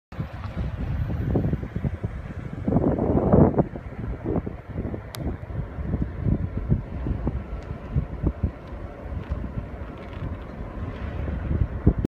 Valley fire from Hidden Valley Lake
Just 20 minutes later, the spot where this video was filmed caught fire. This spot would ultimately become just one of the more than 75,000 acres burned by the Valley Fire on September 12, 2015